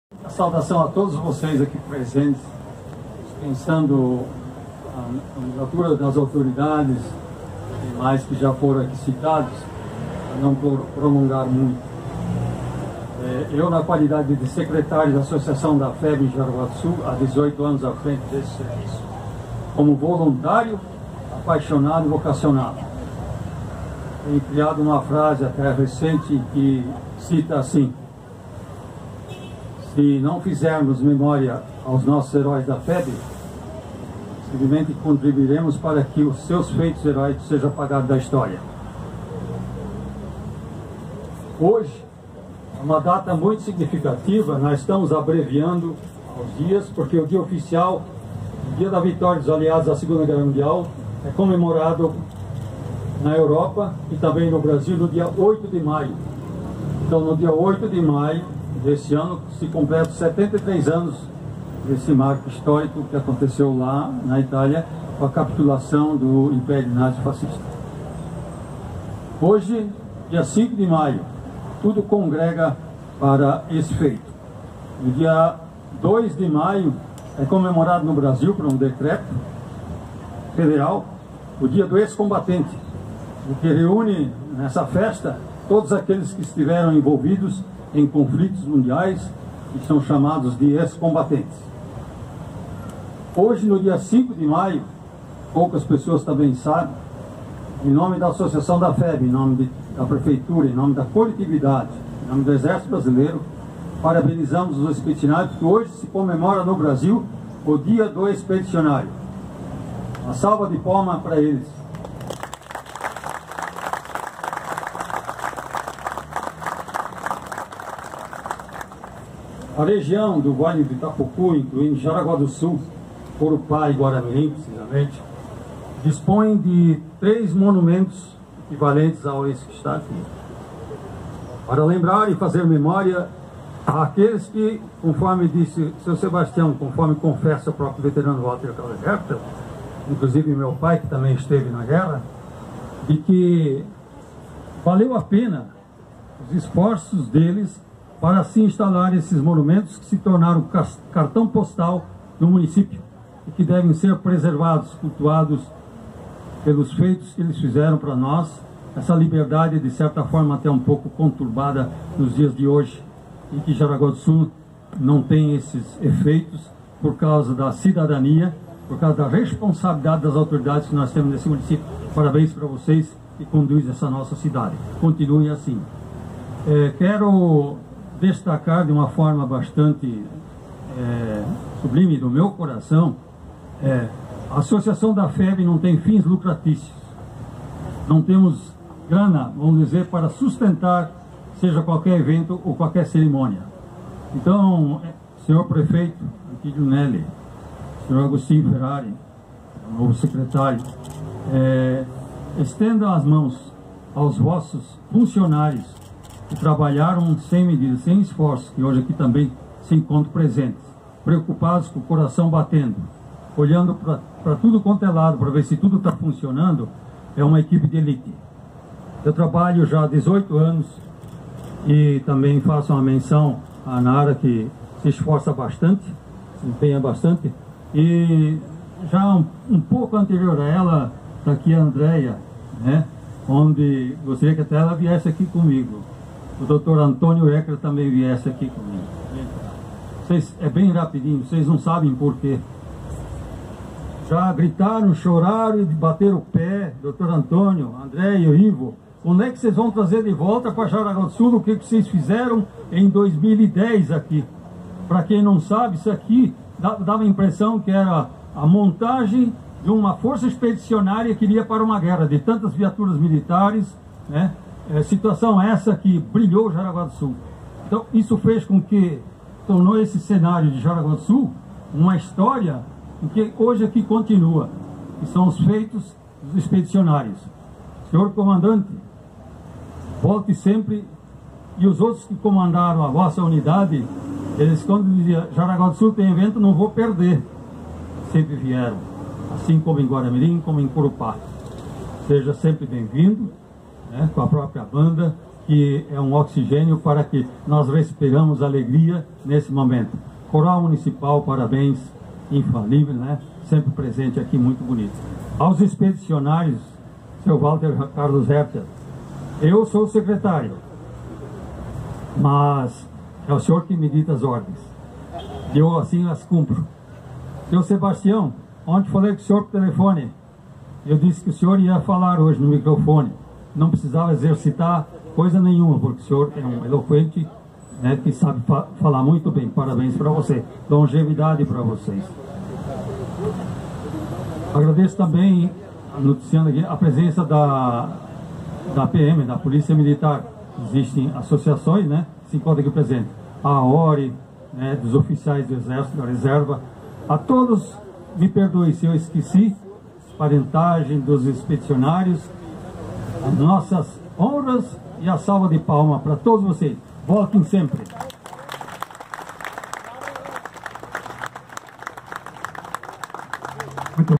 2018 - Anvfeb _ Dia da Vitória – 09 - Ivo Kretzer
A Associação Nacional dos Veteranos da Força Expedicionária Brasileira (Anvfeb), secional de Jaraguá do Sul realizou em parceria com Prefeitura de Jaraguá do Sul (SC), em 05 de maio, sábado, 10h, evento de celebração do Dia do Expedicionário  e Dia da Vitória, que será comemorado em 08 de maio, exatos 73 anos do encerramento da II Guerra Mundial.
O evento trouxe ao Monumento da Anvfeb e Praça do Expedicionário, associativistas, comunidade, imprensa, professores, autoridades militares e civis e famílias remanescentes dos Pracinhas de guerra.
A musicalidade e canto coral do evento foram por conta da Banda do 62º BI de Joinville e Coral Municipal de Jaraguá do Sul.
A seguir serão elencadas algumas autoridades pela importância e respeito ao movimento da Anvfeb:

Reinaldo Sótão Calderaro , tenente-coronel, 62º BI, de Joinville ;
Walter Carlos Hertel, ex-Expedicionário e presidente da Feb de Jaraguá do Sul;
Ivo Kretzer, secretário executivo da Anvfeb;
Sebastião Menott Nunes, ex- Expedicionário da FEB, de Joinville;
Augustinho Ferrari, Secretário Municipal de Esporte Lazer e Cultura, de Jaraguá do Sul;
Anderson Kassner, Presidente da Câmara de Vereadores, de Jaraguá do Sul;
Dieter Janssen, ex-prefeito, de Jaraguá do Sul.
Mestre do cerimonial, Sidnei Marcelo Lopes.